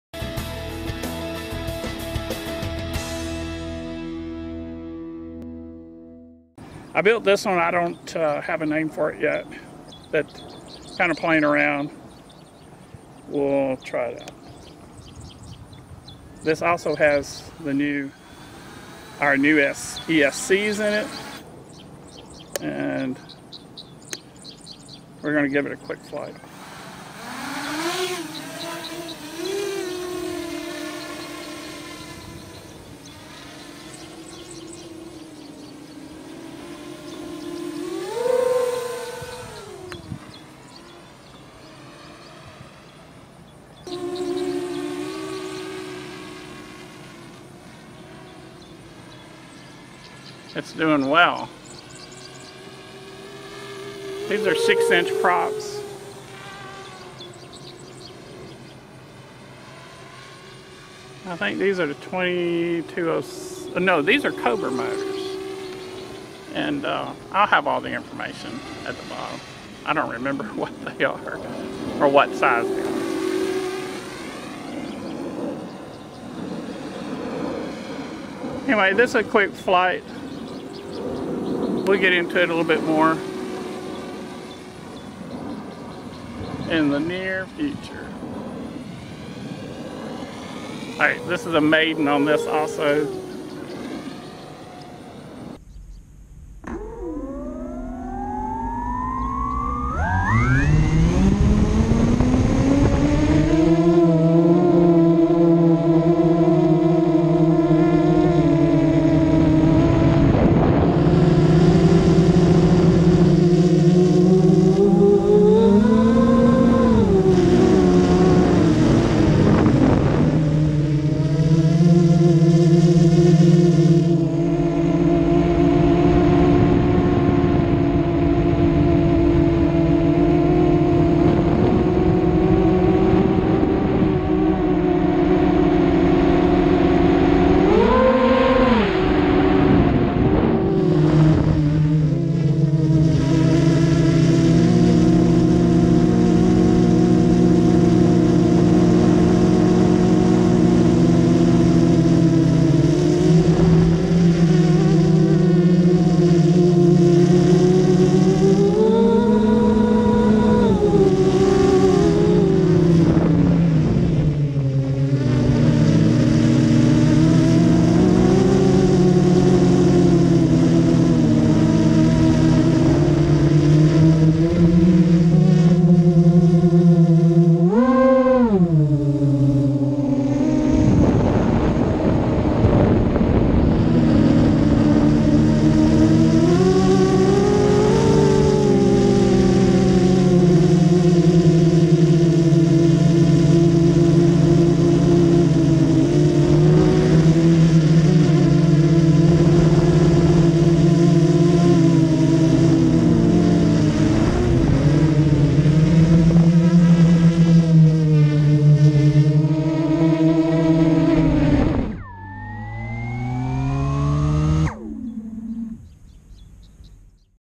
QuadLugs - new build (red & black)
a real fun build - and fly's crazy good - more to come

285mm
SunnySky motors 2207/2100kv
our new Spedix 4in1 esc
0545 props
MultiStar 1400 3S battery